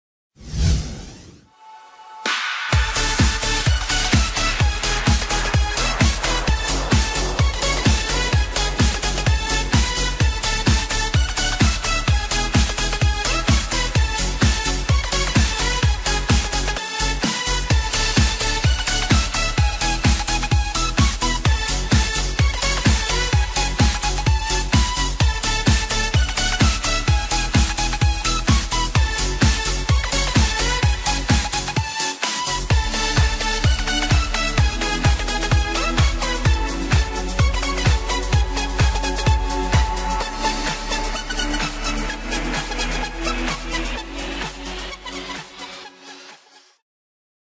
Denon Dj prime 4 Review (Español)
Le echamos un vistazo a La Denon Dj Prime 4 el controlador Dj que está dando de qué hablar!

¿Qué piensas de este controlador?

¿Estás interesado en cursos avanzados?
En la escuela Dj Beats estamos para convertirte en el mejor DJ!